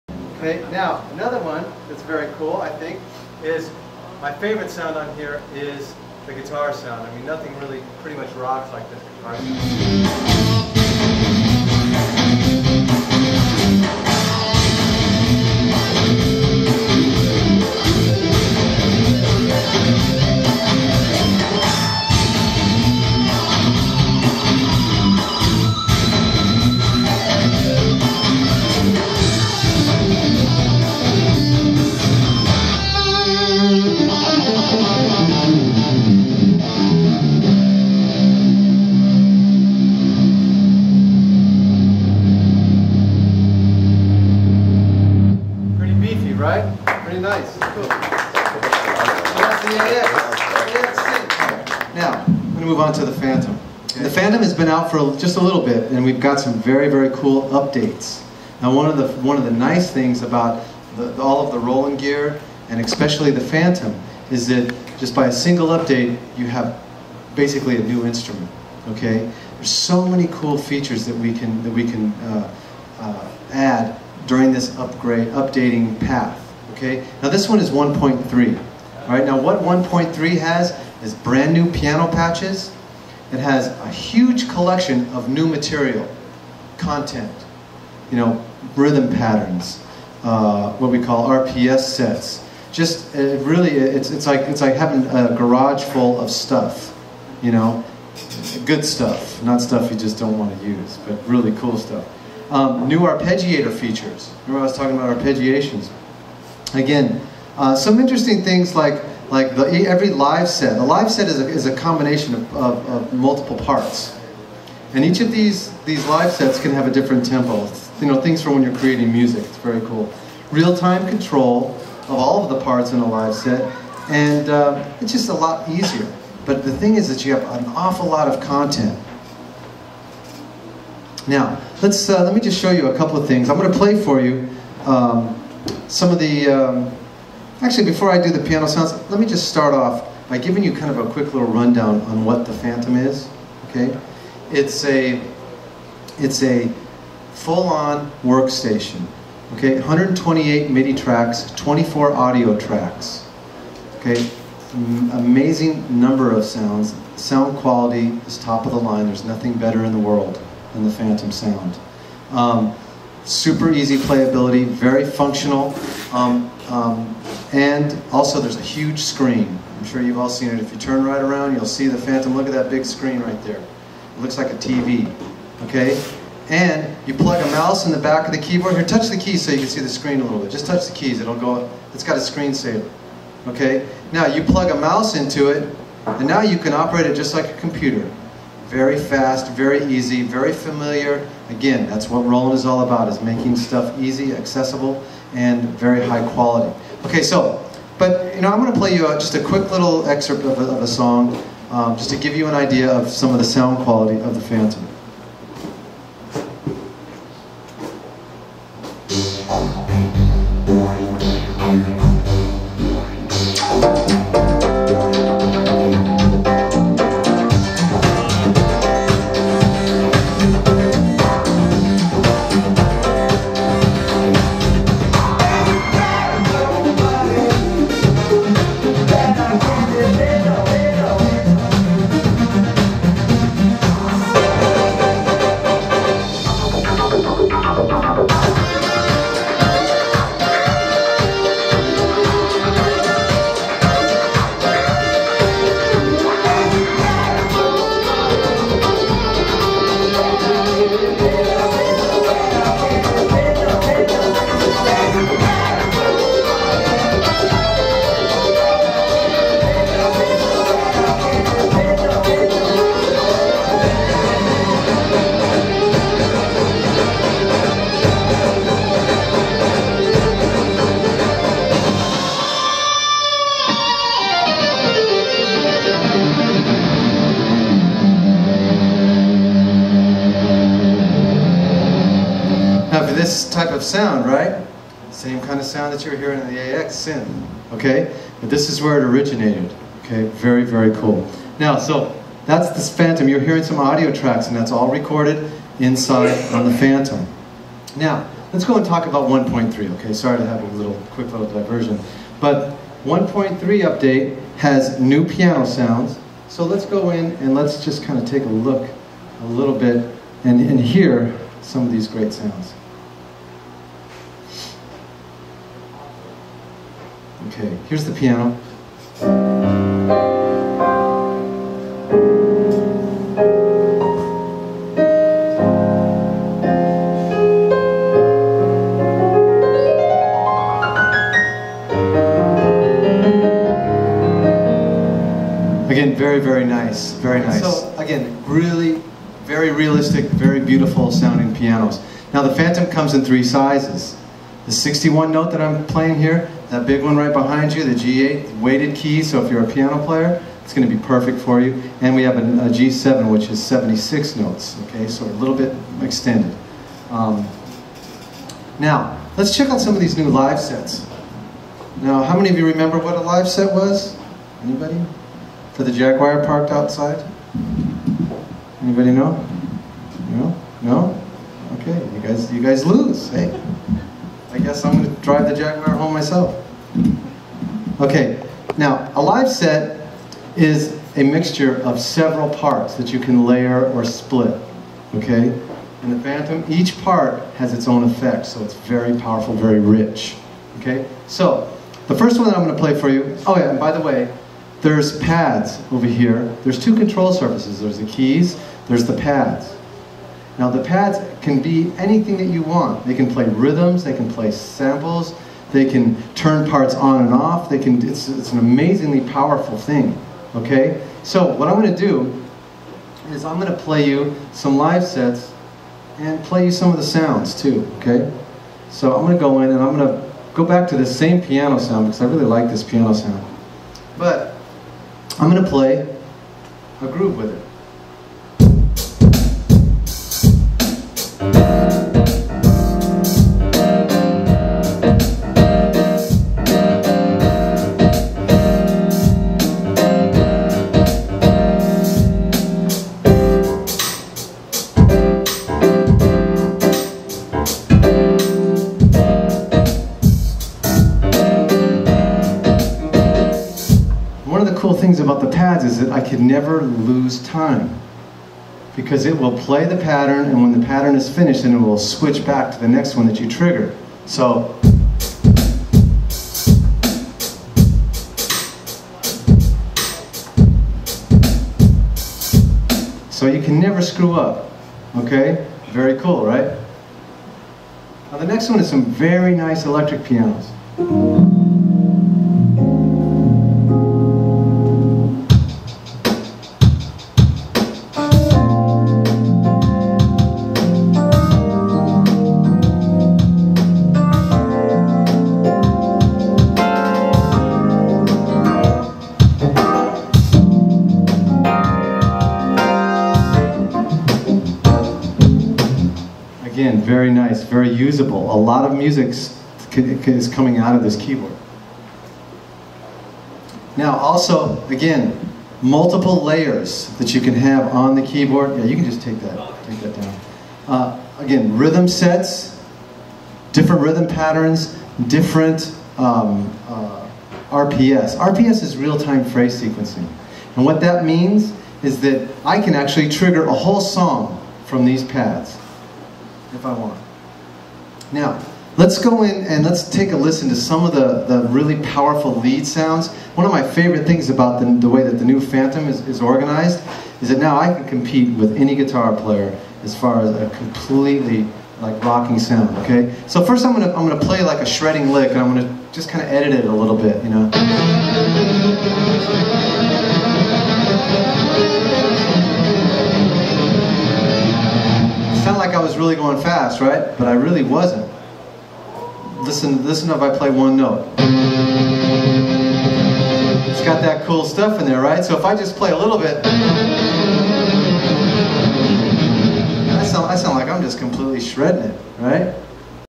5/8 - Roland Clinic
John Hernandez and Scott Tibbs showing the latest Roland gear

TD-20SX
Fantom G
AxSynth
JunoDI